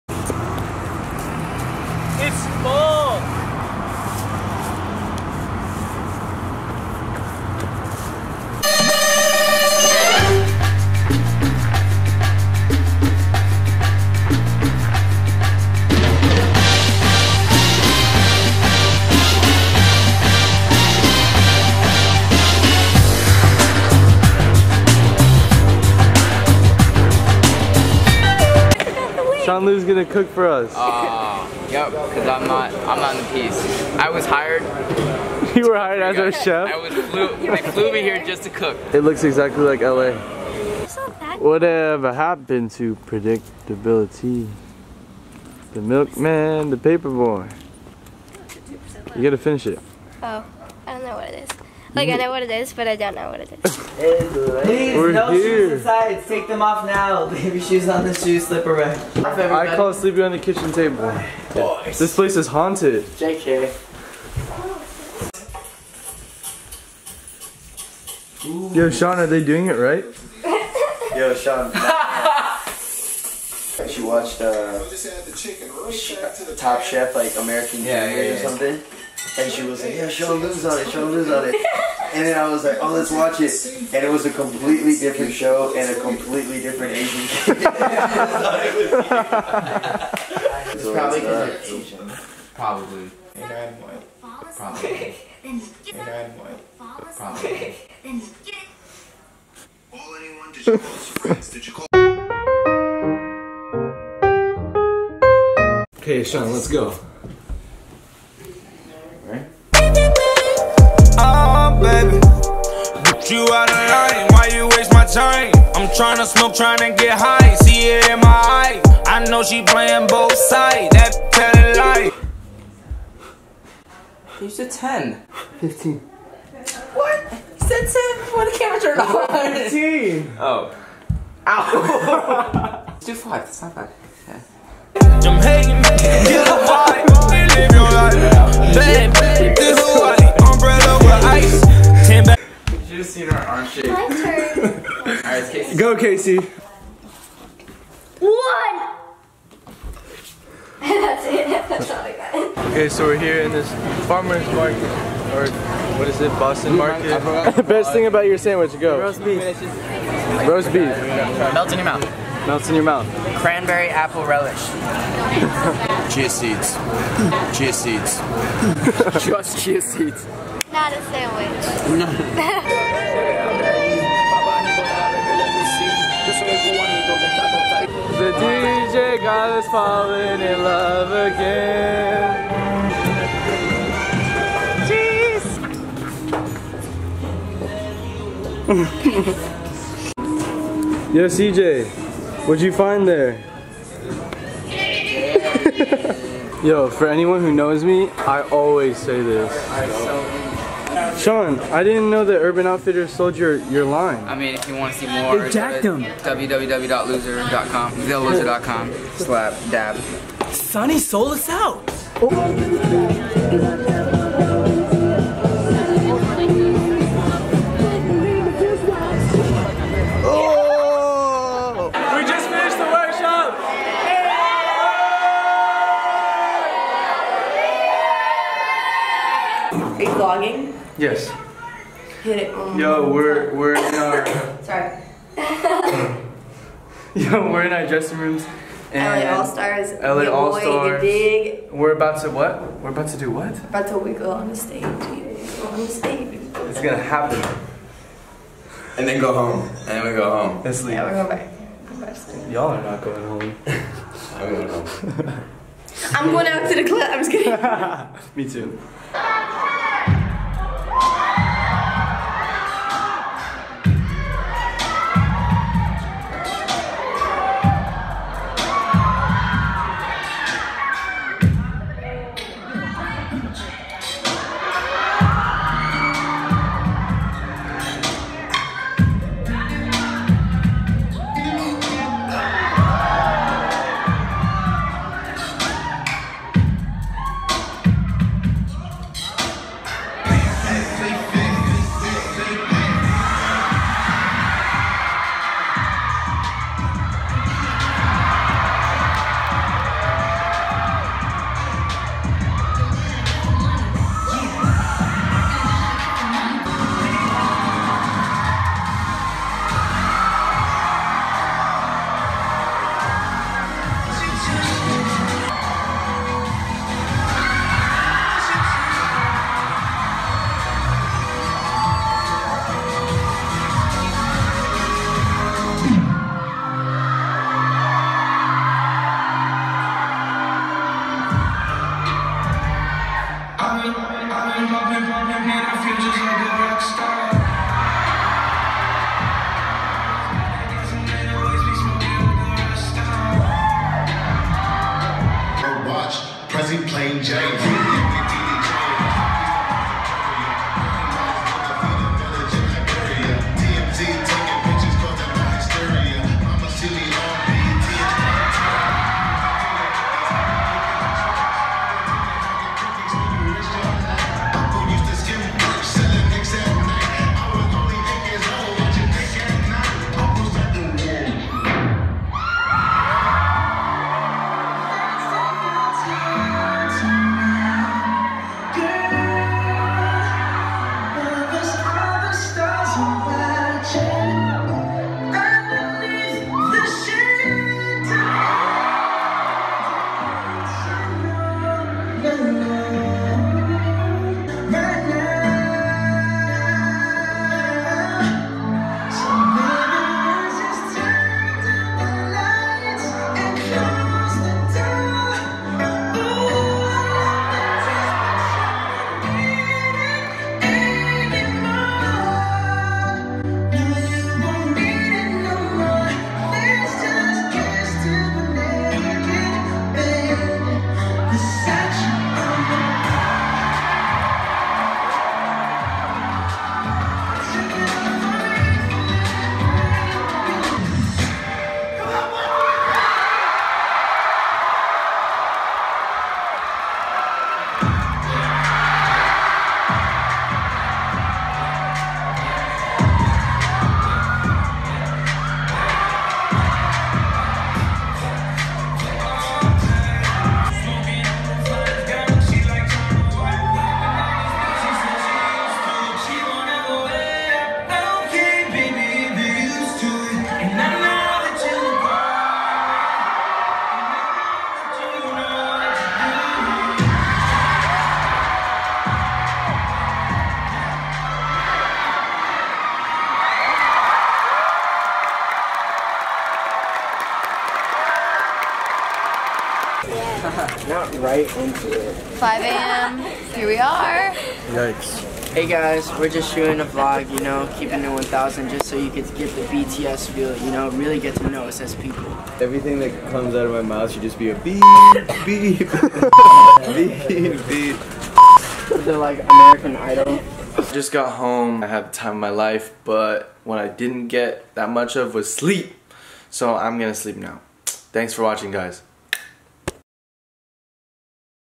Boston with the Wiggle Crew / Vlog
Went to Boston with a few AMAZING human beings. Thank you Alexander Chung! Thank you FUNKTION 9 for having us. Couldn't have had a better time. :)

J2911